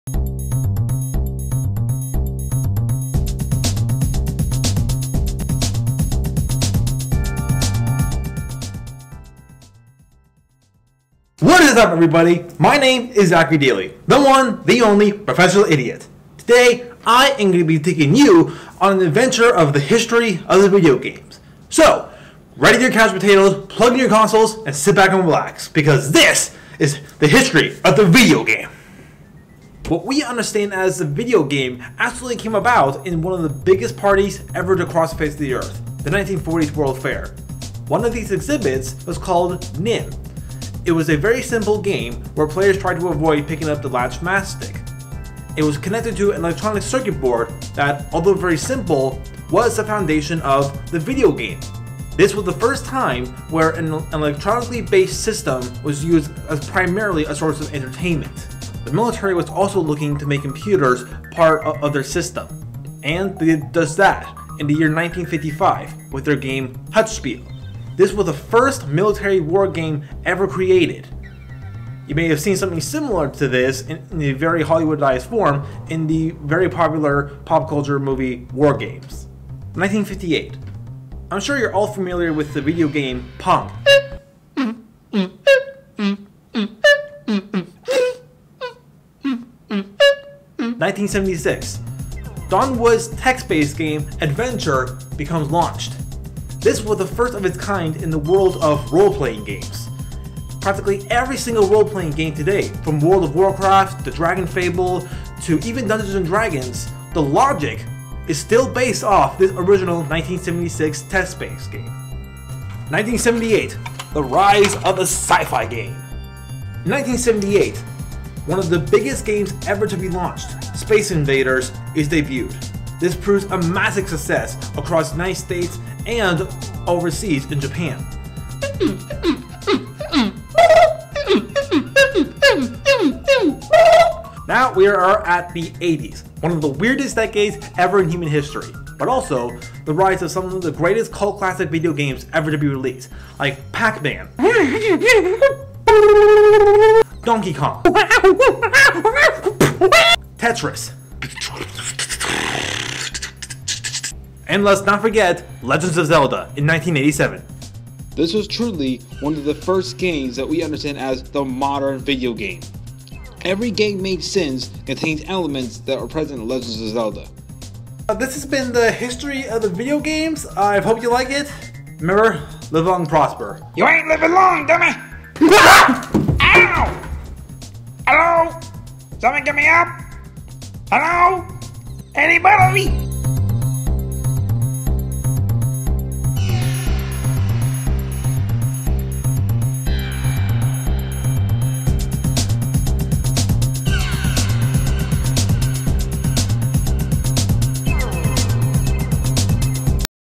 A Short History of the Video Game
My final assignment for my Spring 2017 Motion Graphics class at CSU East Bay.